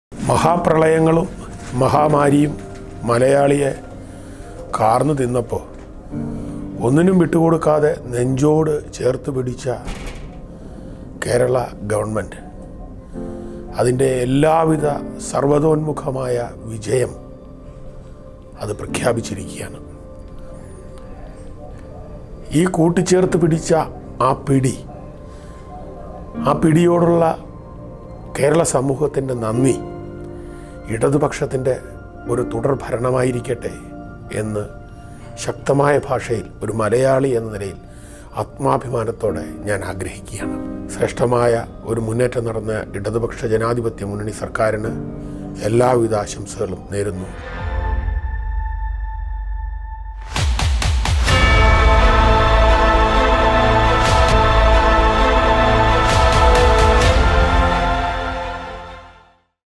പ്രതിസന്ധികളിൽ ചേർത്തു പിടിച്ച സർക്കാരാണിത് - നടൻ അനൂപ് ചന്ദ്രൻ
മഹാമാരികളും മഹാപ്രളയങ്ങളും ആഞ്ഞടിച്ചപ്പോളും പ്രതിസന്ധികളിൽ തളരാതെ കേരളജനതയെ ചേർത്തു നിർത്തിയ സർക്കാരാണിത് - എൽഡിഎഫ് സർക്കാരിനെക്കുറിച്ച് നടൻ അനൂപ് ചന്ദ്രൻ.